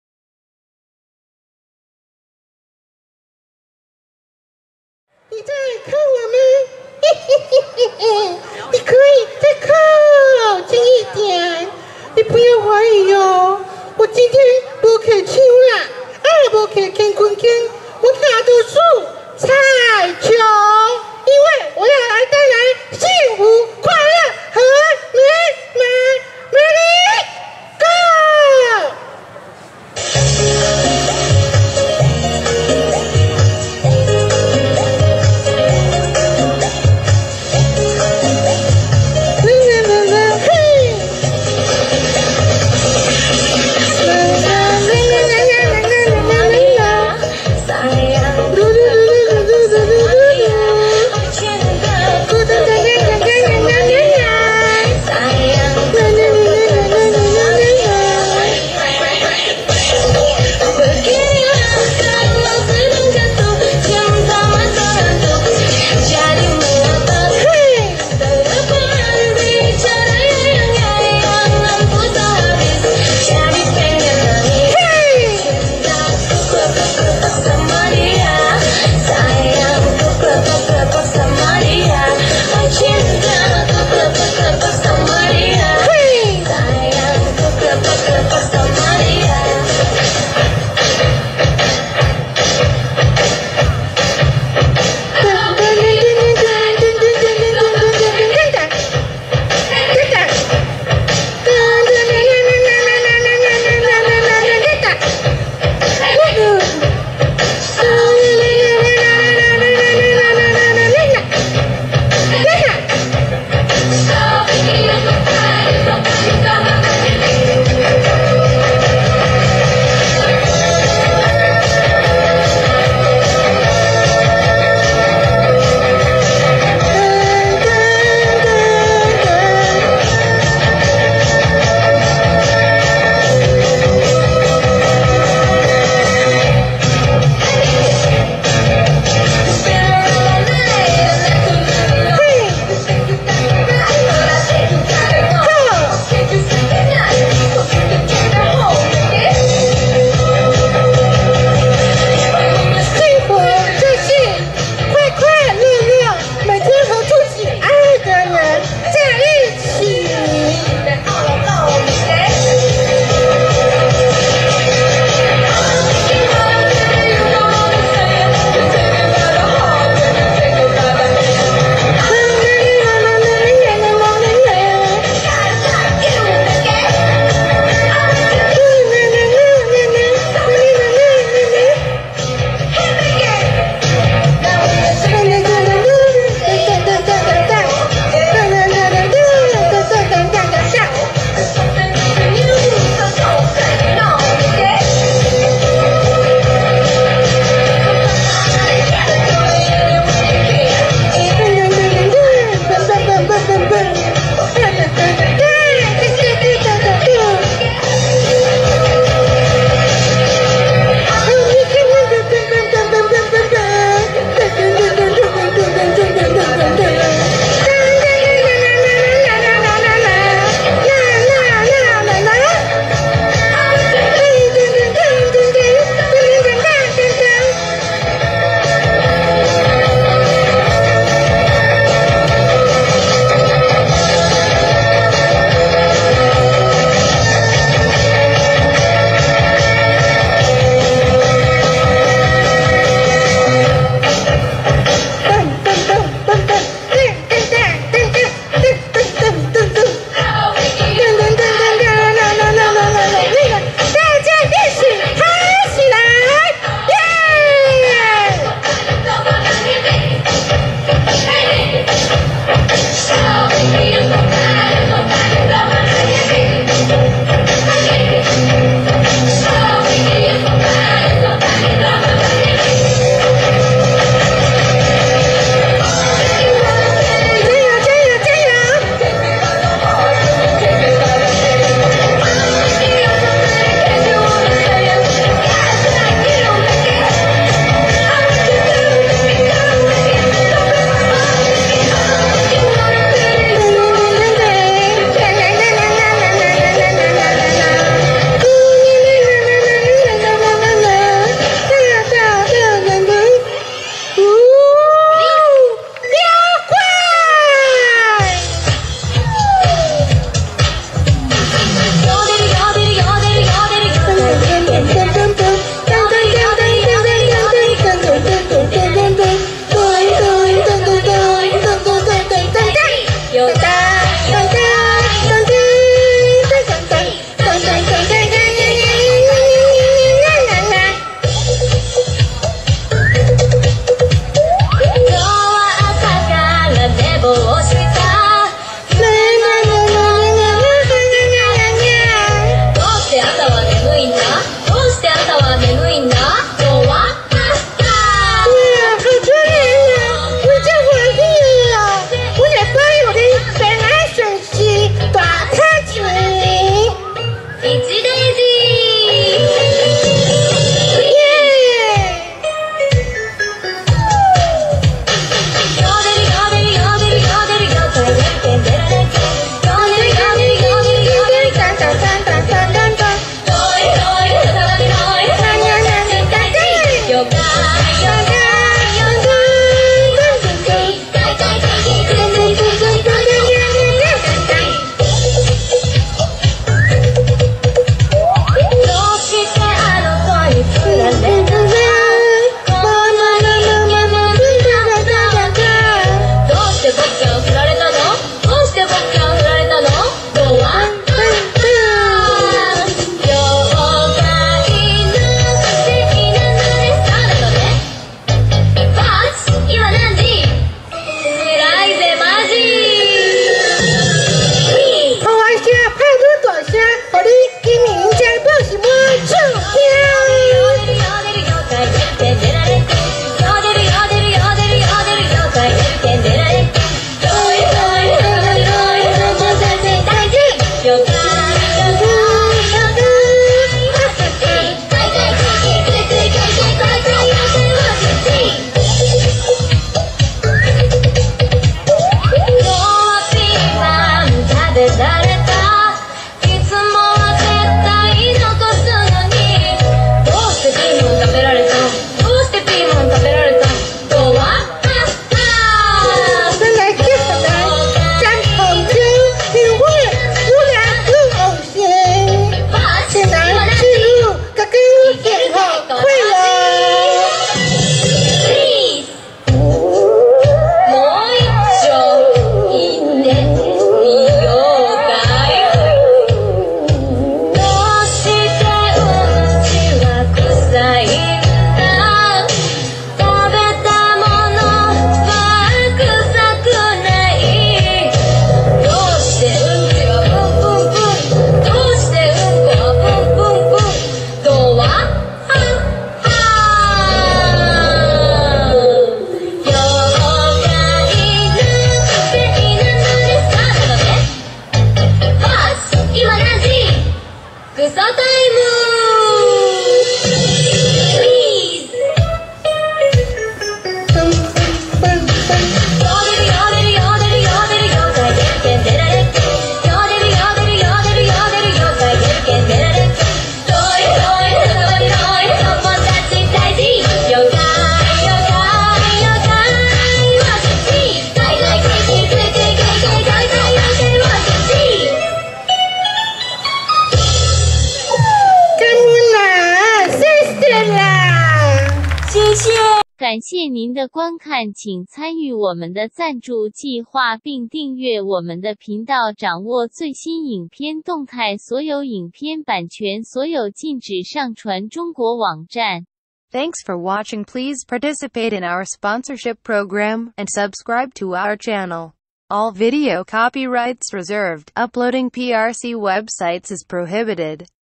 電音三太子大賽 南投廣靈民俗技藝團(4K HDR)@2016高雄幸福三太子[無限HD] 🏆
本影片未開營利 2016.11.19 高雄三鳳宮